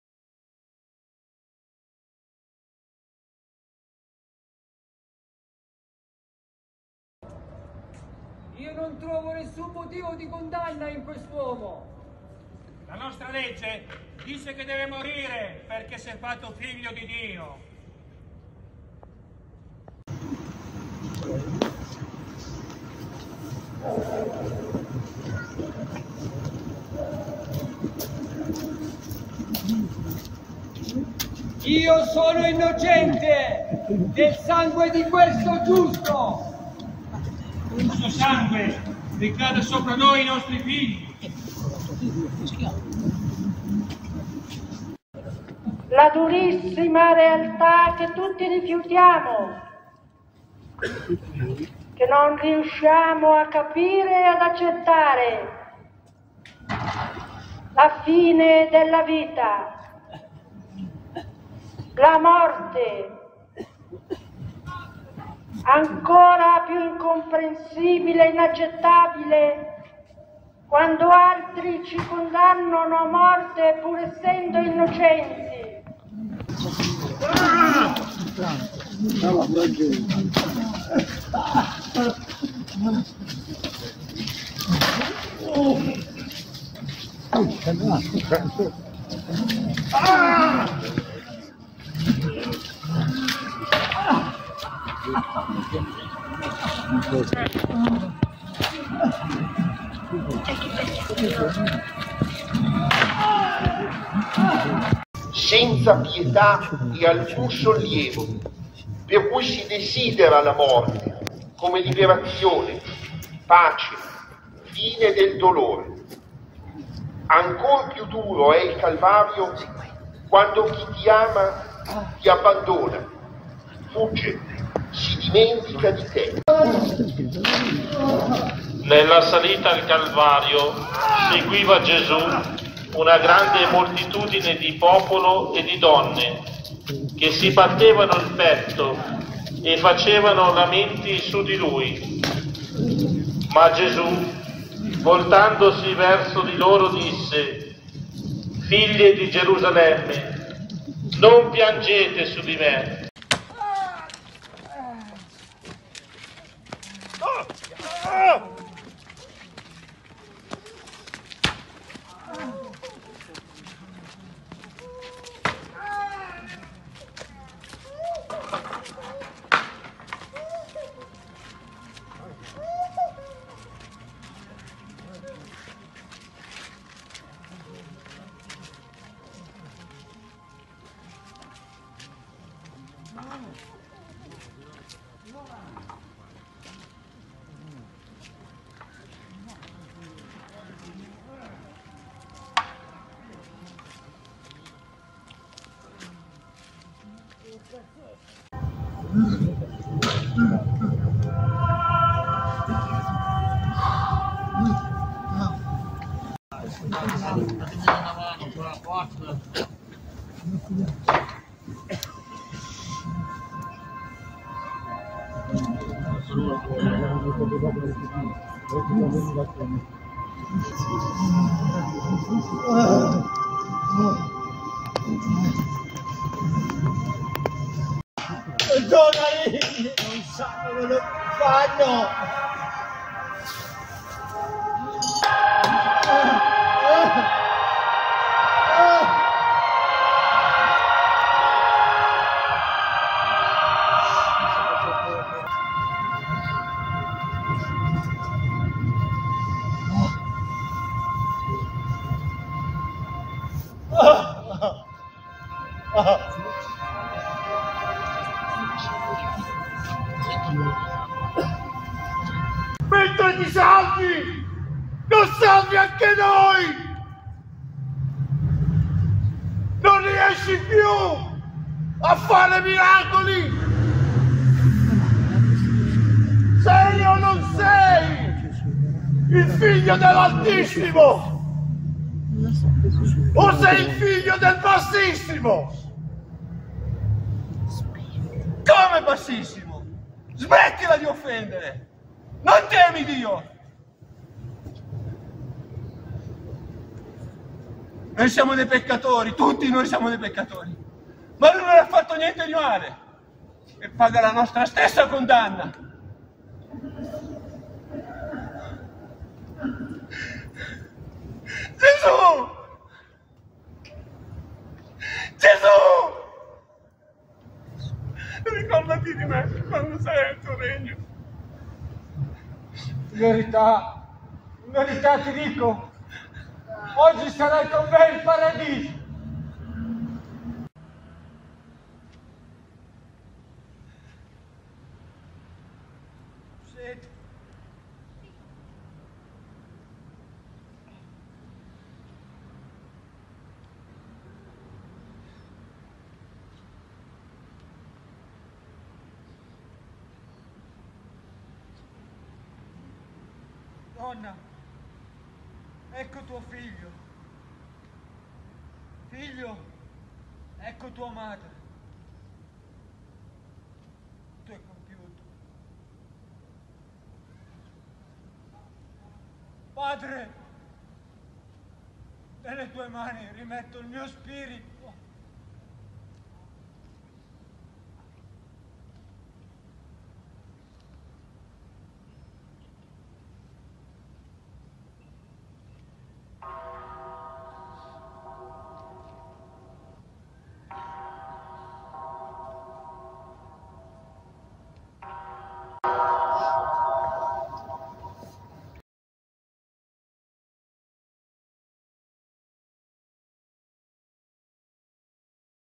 Via Crucis vivente anno 2024
Ogni Venerdi' Santo a Vimodrone(Mi),c'e' la via crucis vivente.